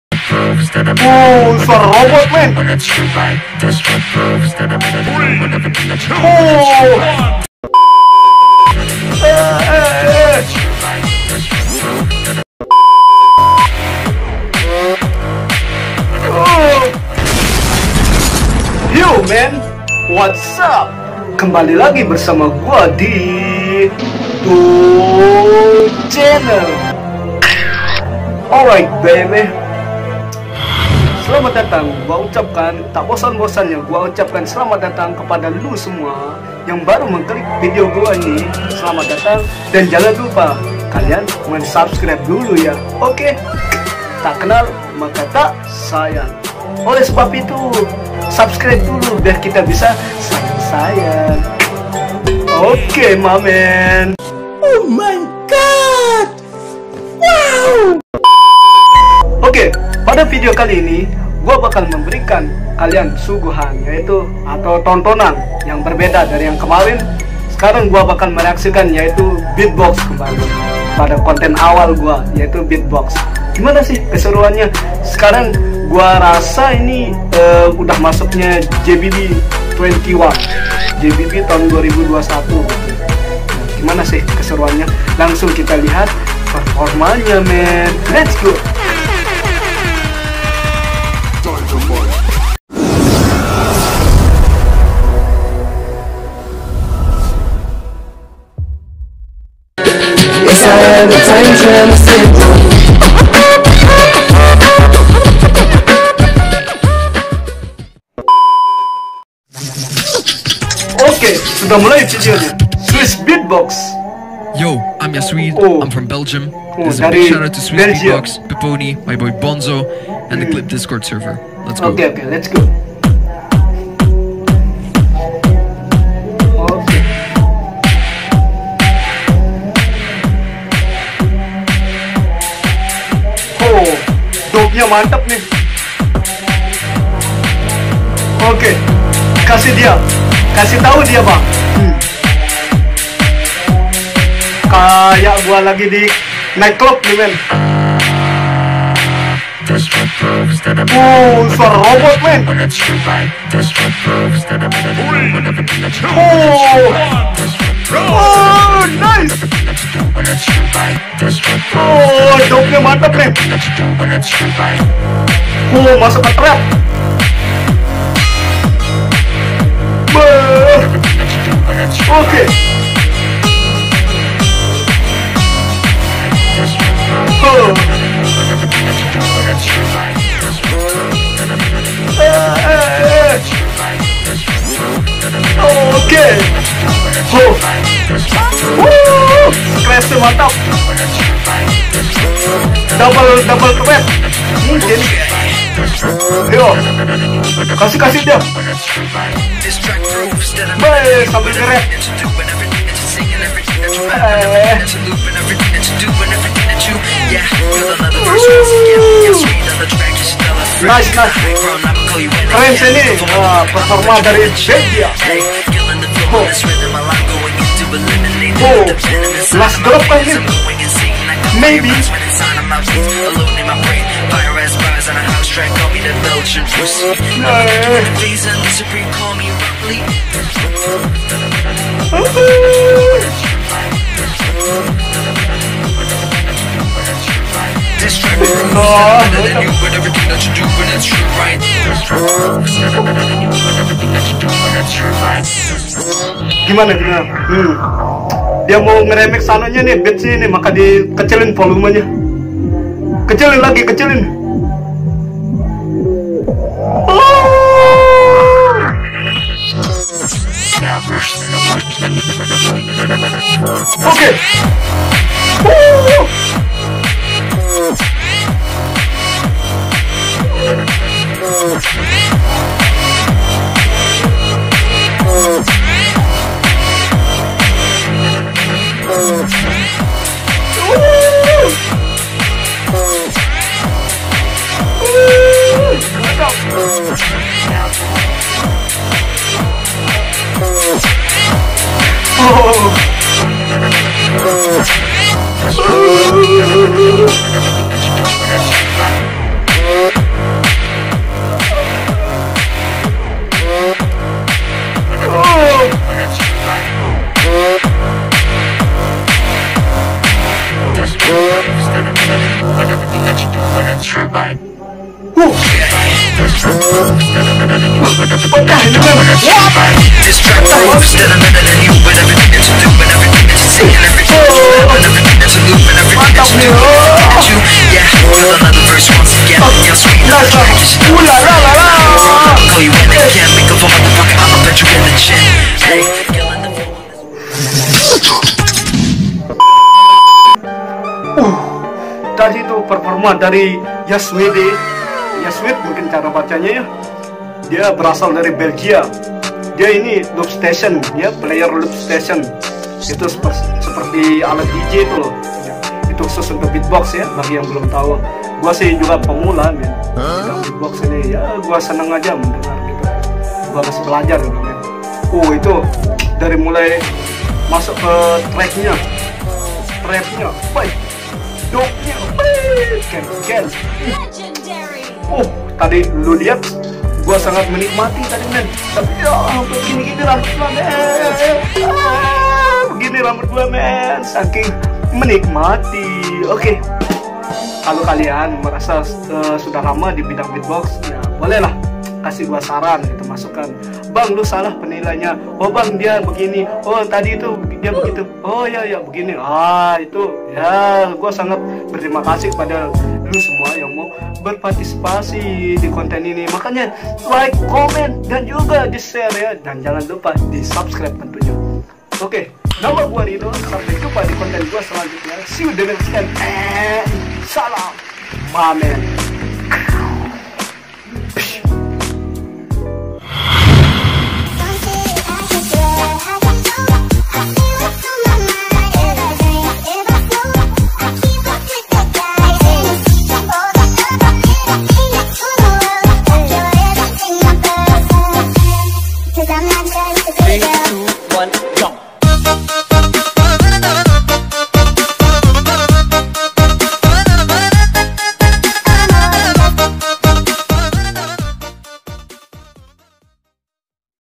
LAGU DUGEM DARI BELGIA 🤘🏻🙂
Kali ini kita akan menikmati segmen yang sangat kalian nanti² yaitu BEATBOX, banyak diantara kalian yg request ke gw baik itu secara langsung maupun virtual..
oke tanpa banyak bacot kyk bekicot, langsung saja deh elu semua tonton video yg gw buat ini ya.. !! 😀
Alright Ma Men, Enjoy For Watching My Video 🤘🏻😎